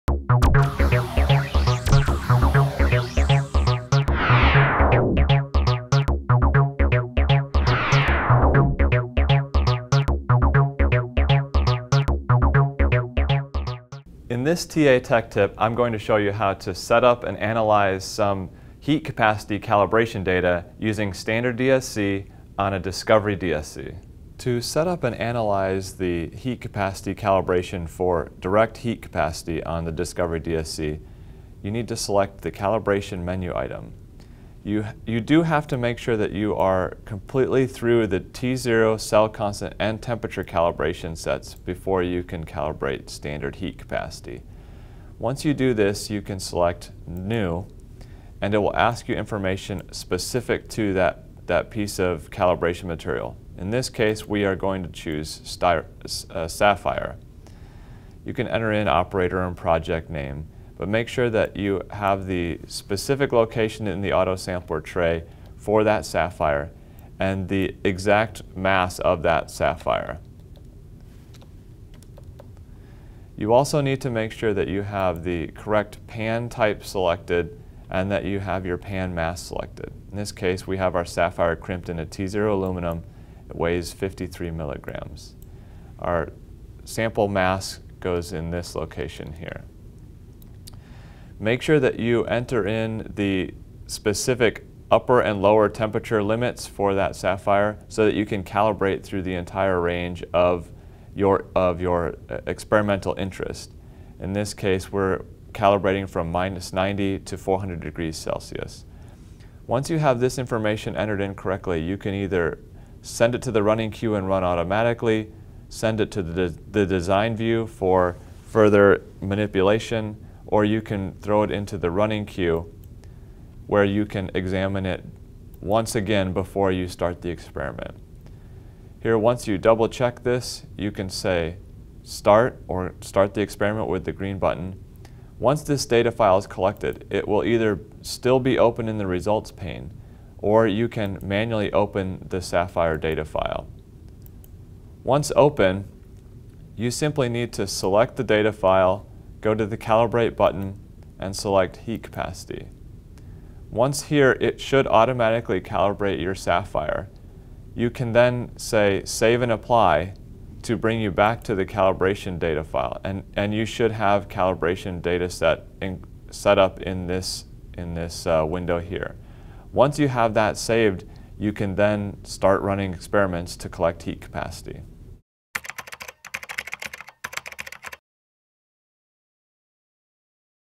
TRIOS - Standard Heat Capacity DSC
This Tech Tip will show you how to run a Standard Heat Capacity Calibration using Trios for your Discovery DSC system.